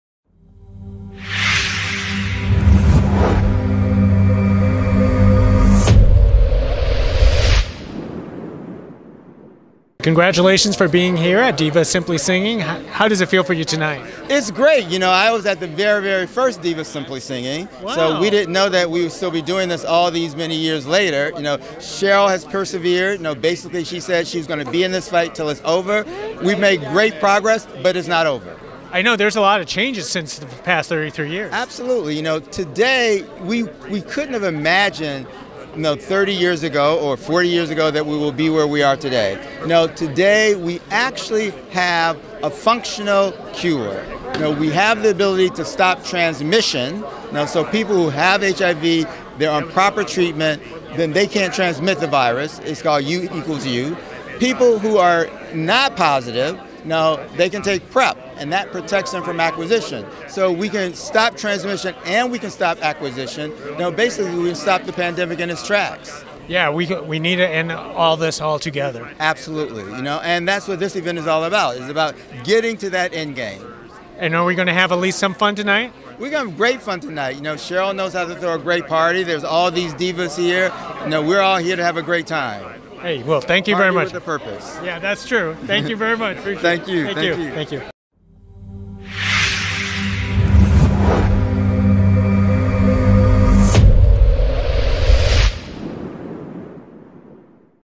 Phill Wilson Carpet Interview at Divas Simply Singing! 2023
LRM Online's Gig Patta spoke to activist Phill Wilson on the red carpet prior to the event for Divas Simply Singing!

Now in its 33rd year, DIVAS Simply Singing! Raising Health Awareness, a health awareness initiative of The DIVA Foundation, was originally founded as a living memorial to the many friends Sheryl Lee Ralph lost during the AIDS Crisis. DIVAS Simply Singing! has grown into the longest-running health awareness benefit concert in the US that raises awareness and funds for programs that empower and educate audiences to own and advocate for their health and the health of others.